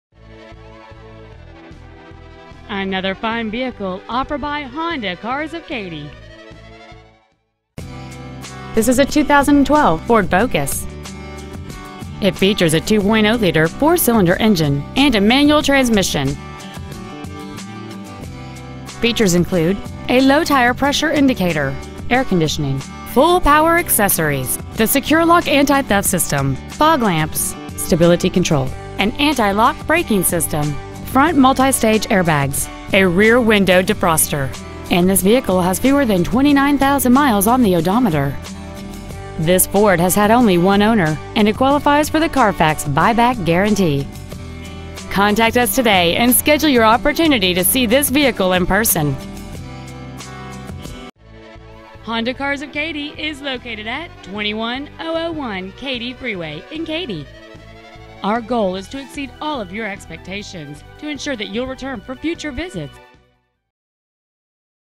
2012 Ford Focus Spring TX
Year : 2012
 Make : Ford
 Model : Focus
 Engine : 2.0L GDI I4 flex fuel engine
 Trans . : Manual
 Exterior : Silver
 Miles : 28,287

 2012 Ford Focus Katy TX